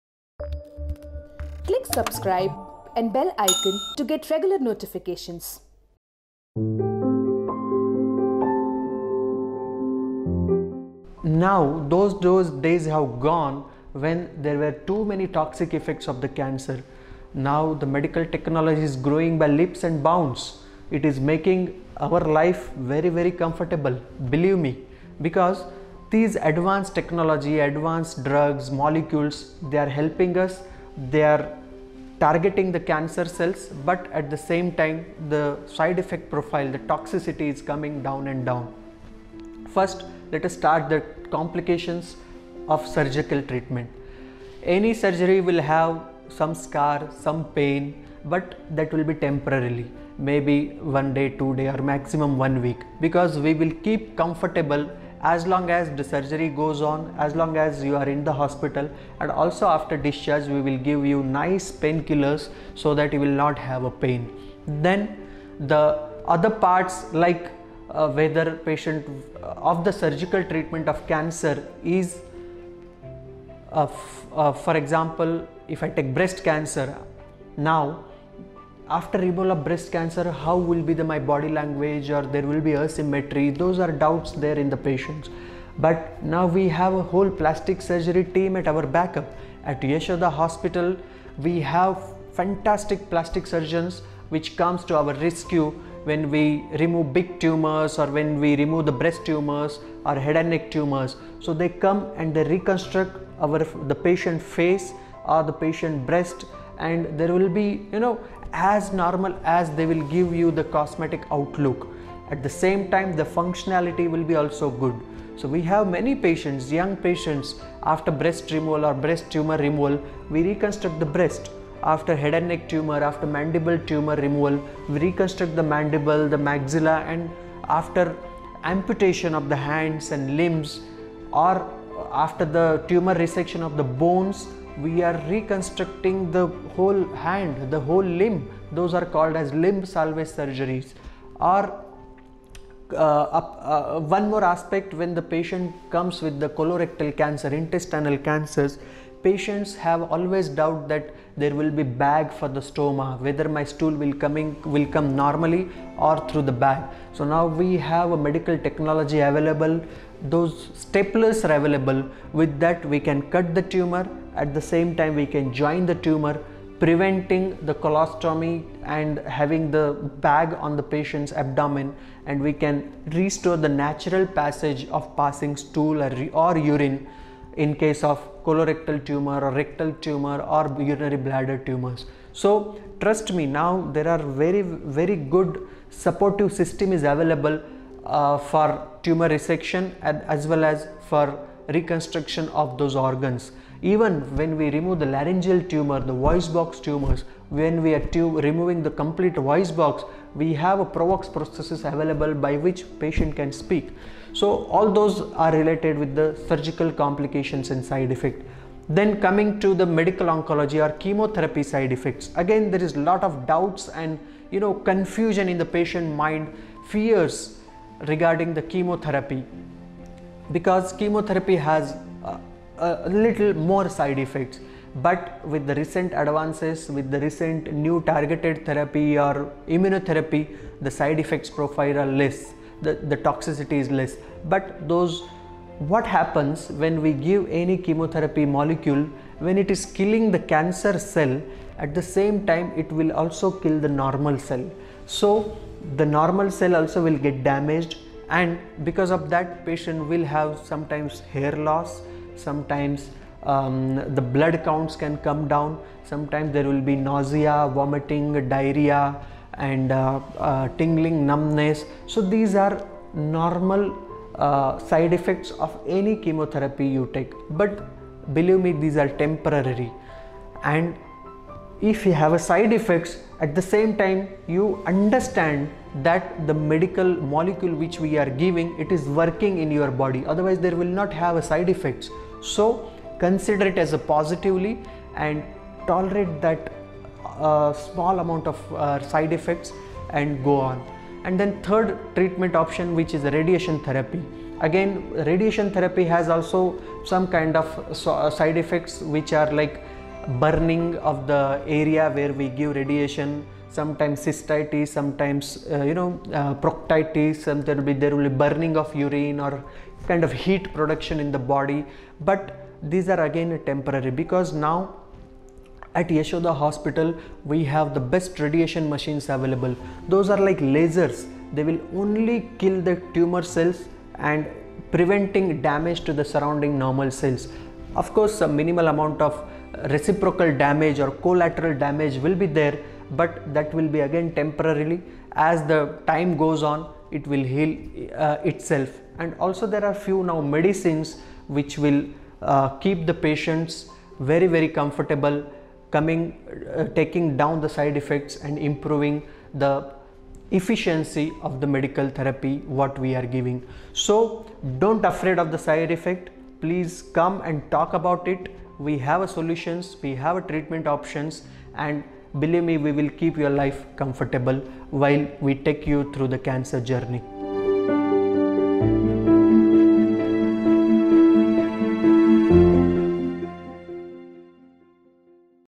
Cancer Treatment: Medical Technology Role in Reducing Side effects
Cancer Treatment: Why do patients suffer from hair loss and nausea after undergoing chemotherapy? Do all forms of cancer treatment have such side-effects?

Dr. Sachin Marda, Consultant Surgical Oncology and Robotic Surgery at Yashoda Hospitals explains the side-effects that can occur due to surgery, chemotherapy, and radiation therapy.

Surgical treatment of cases like breast cancer where mastectomy is required can lead to the partial/complete removal of a body part or blemishes. Support can be provided to the patient through reconstructive surgery.

Chemotherapy and radiation therapy also have side-effects. Patients can suffer from burning of the area, cystitis, proctitis, etc. These complications are generally temporary and can be resolved with immunotherapy and the use of advanced technology.

We have created a series of informative videos on cancer: its signs and symptoms, stages, treatment, and prevention measures.